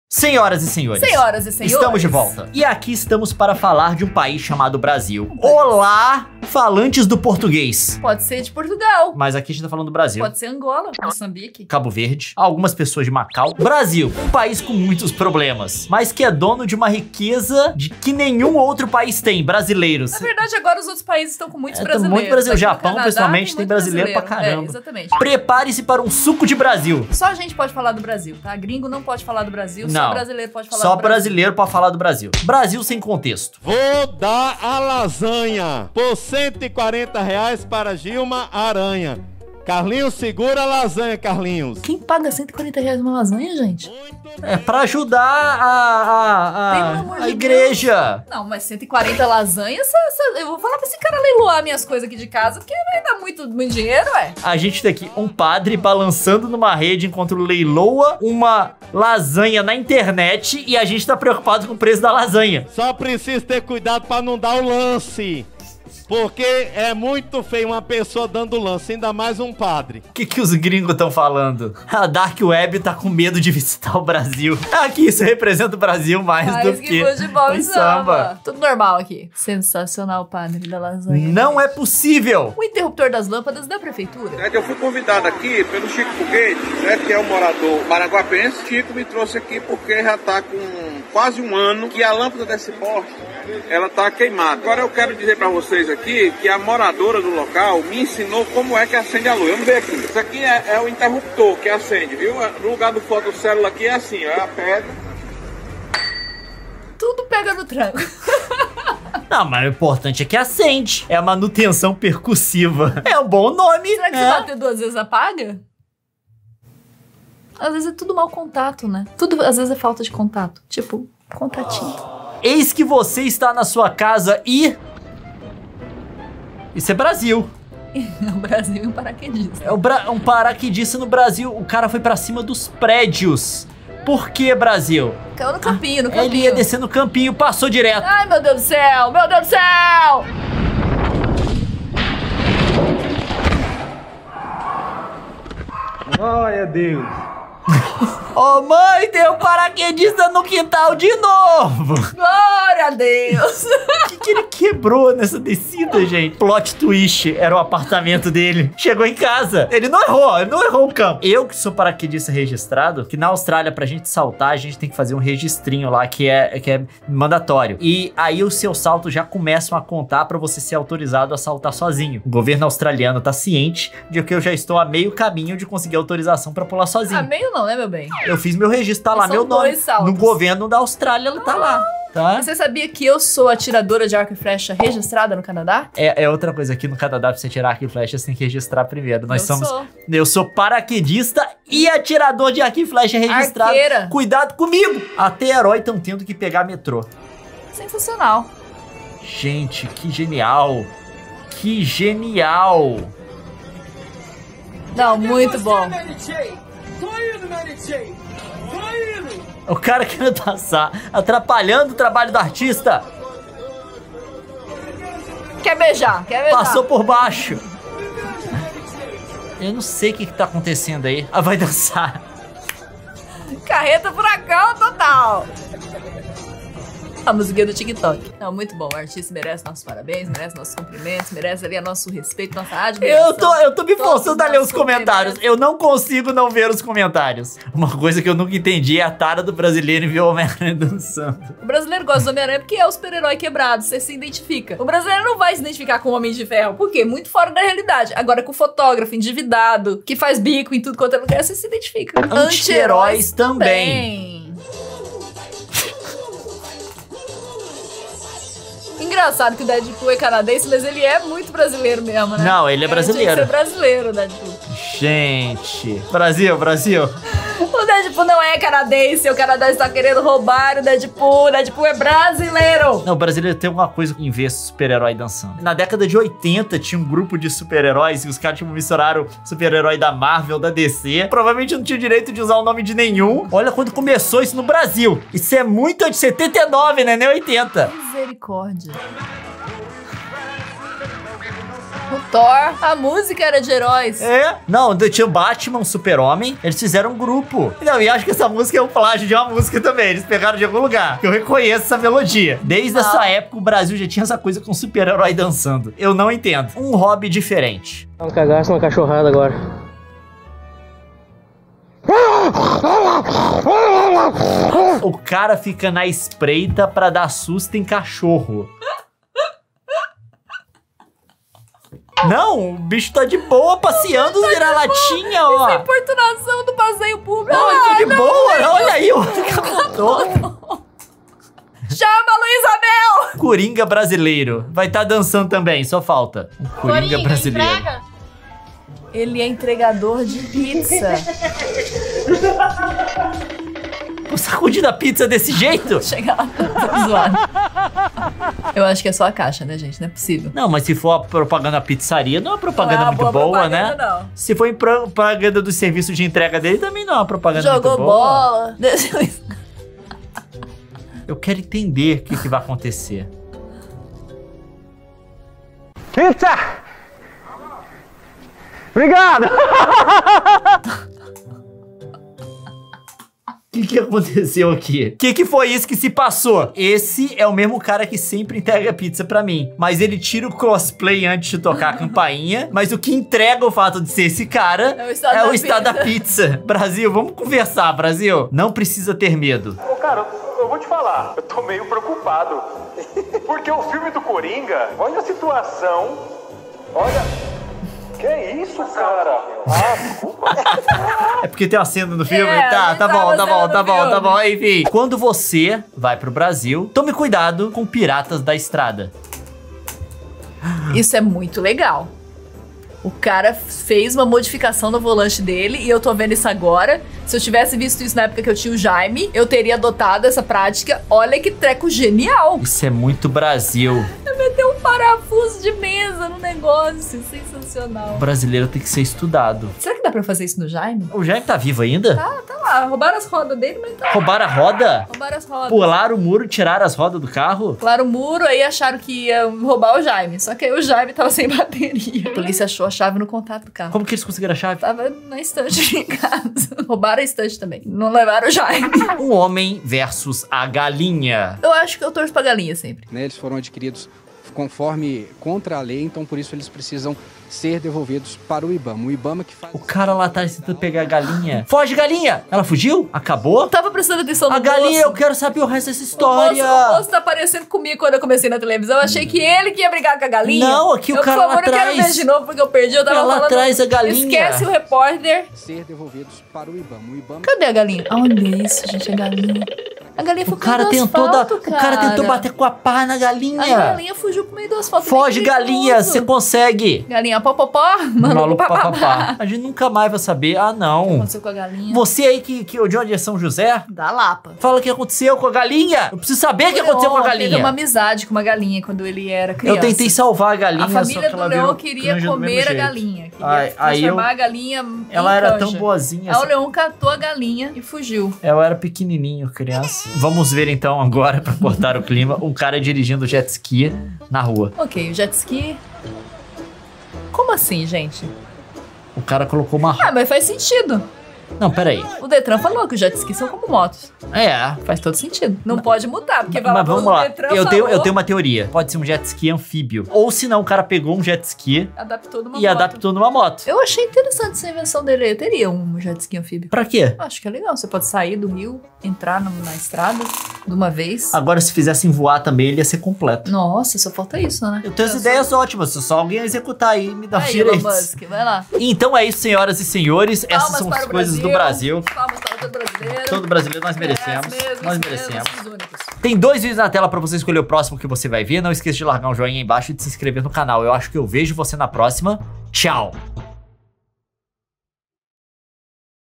UM PAÍS CHAMADO BRASIl
Leon e Nilce reagem a vídeos que só poderiam ter sido gravados no Brasil